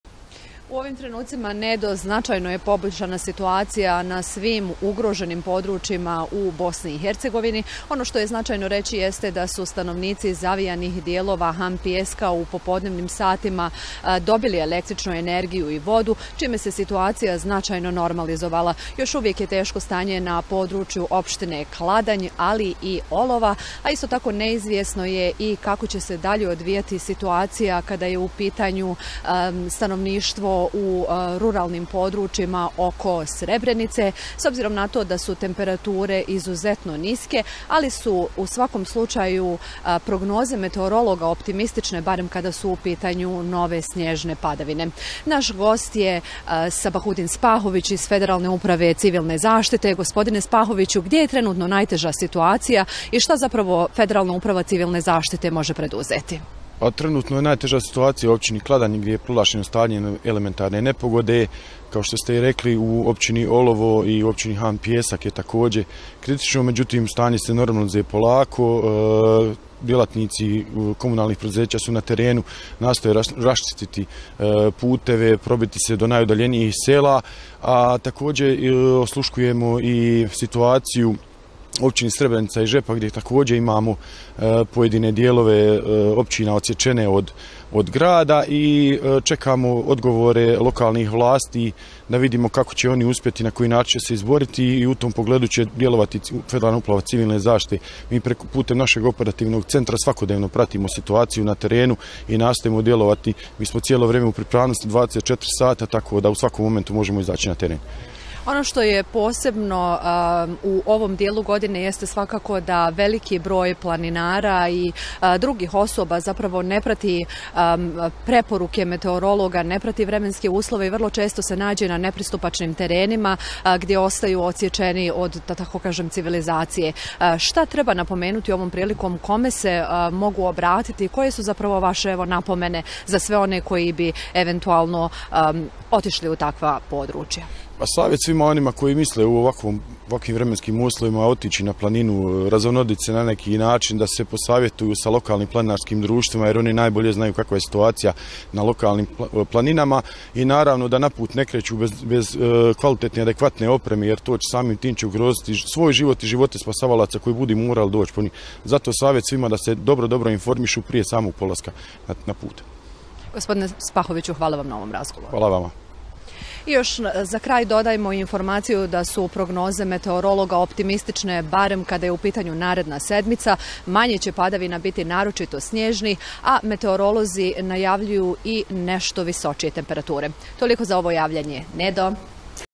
STANOVNICI HAN PIJESKA DOBILI STRUJU I VODU, ČISTE SE LOKALNI PUTEVI
Iz Civilne zaštite su medijima potvrdili da je danas od 12 sati izvorište vode u Han Pijesku dobilo struju, a potom će i oni potrošači, koji su jučer ostali bez vode, dobiti vodu